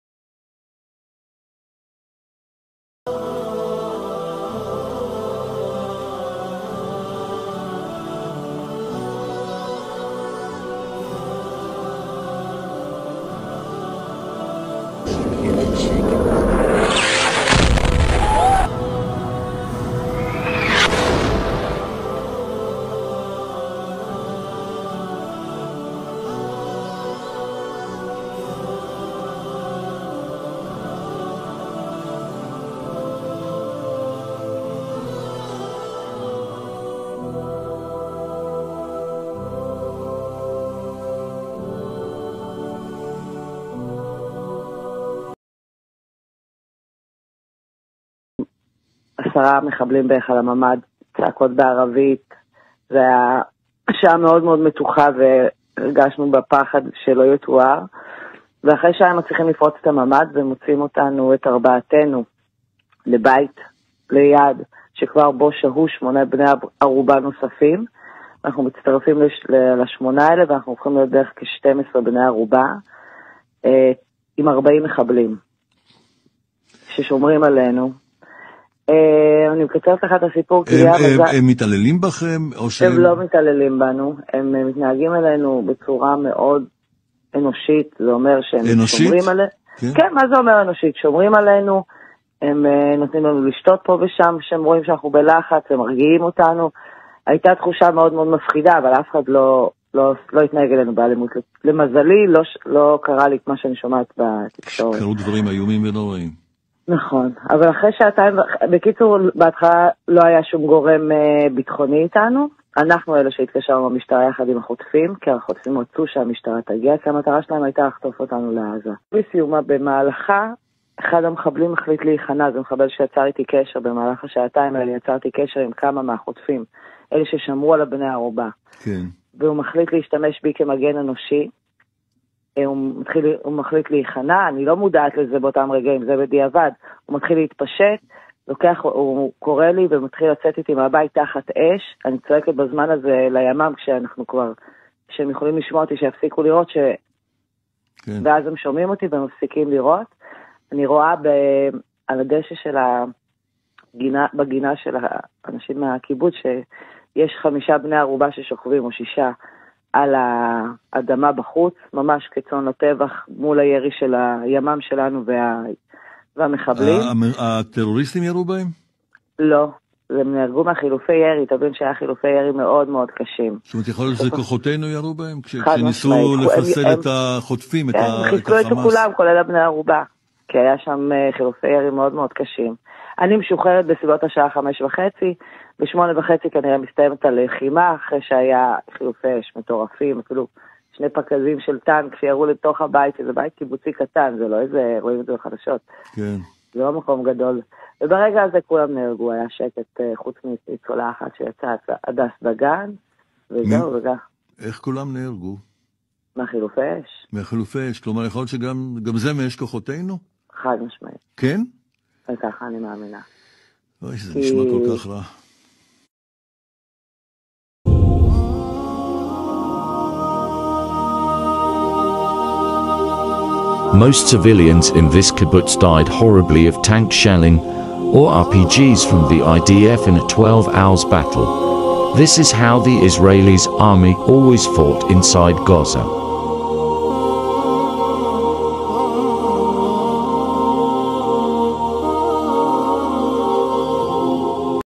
La propaganda sionista contro Gaza
La verità sul bombardamento dell'ospedale di Gaza e della tragedia nei kibbitz israeliani